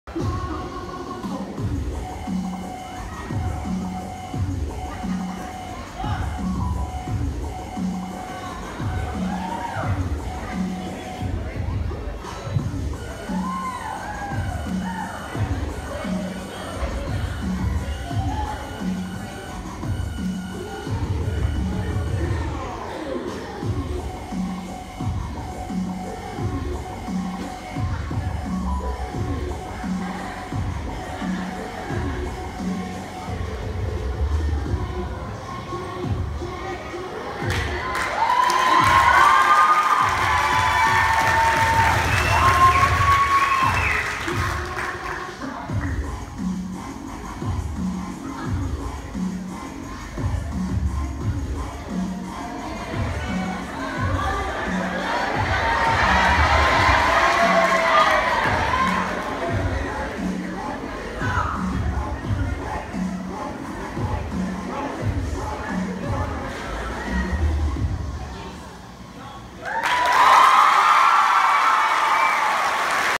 Chiefland 2016 homecoming pow wow
Demetrius Bowers, Jahvel Blair and Abel Days perform at the homecoming pow wow at Chiefland Middle High School.